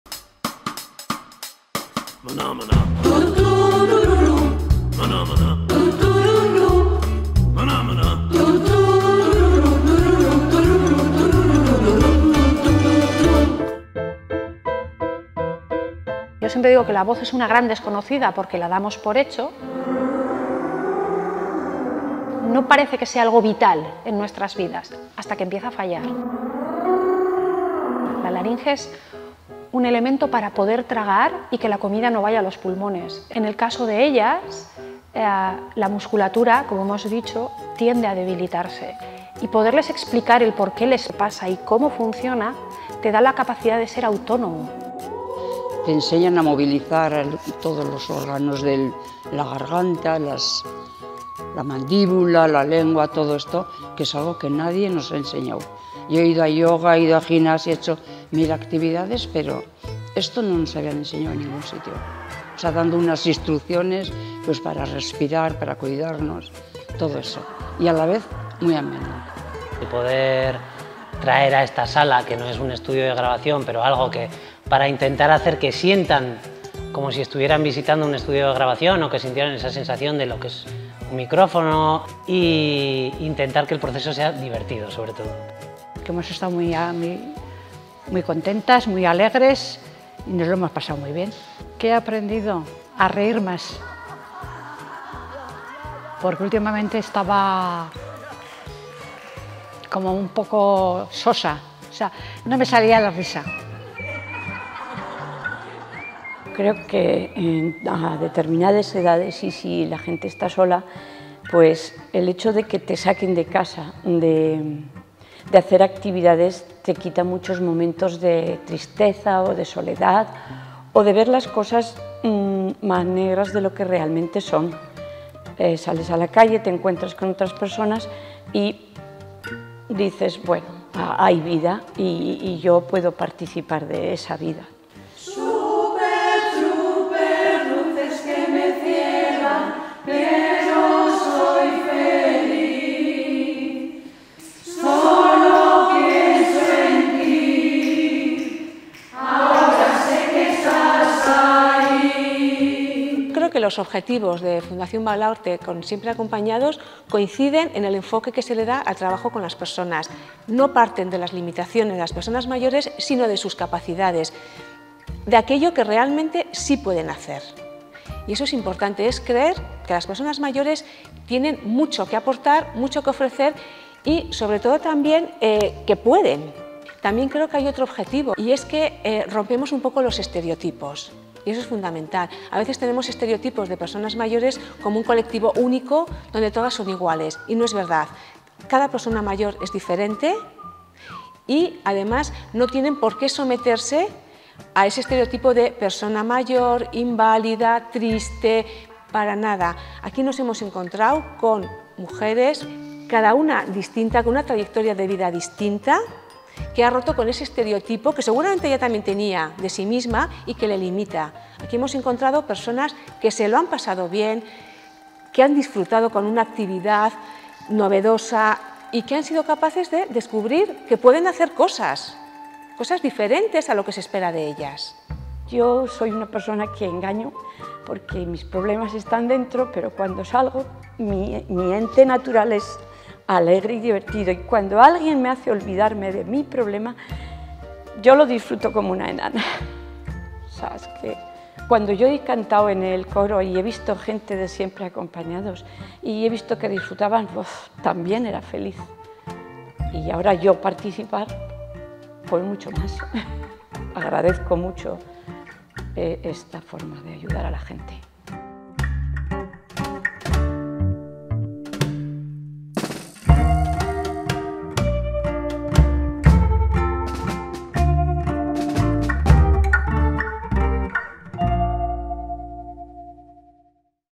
Siempre acompañados. Baluarte a Tono
Vídeo que explica una iniciativa musical de Fundación Baluarte para personas mayores del programa Siempre Acompañados de Fundación "la Caixa" y Fundación Caja Navarra